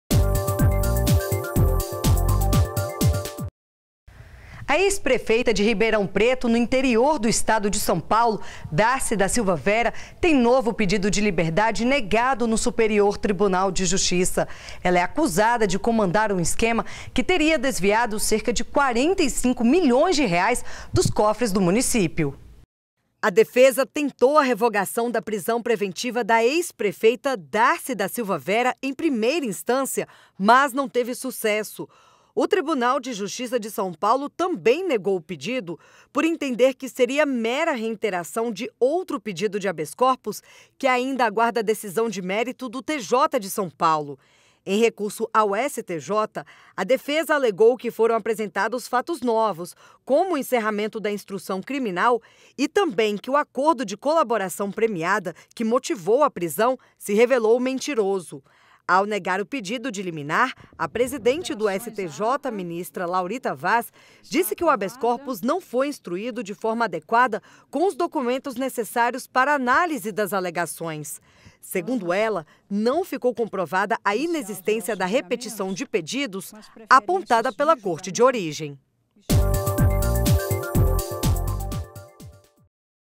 Clique STJ - Ex-prefeita Presa (04/01/2018)
A ex-prefeita de Ribeirão Preto, no interior do estado de São Paulo, Dárcy da Silva Vera, tem novo pedido de liberdade negado no Superior Tribunal de Justiça. Ela é acusada de comandar um esquema que teria desviado cerca de 45 milhões de reais dos cofres do município.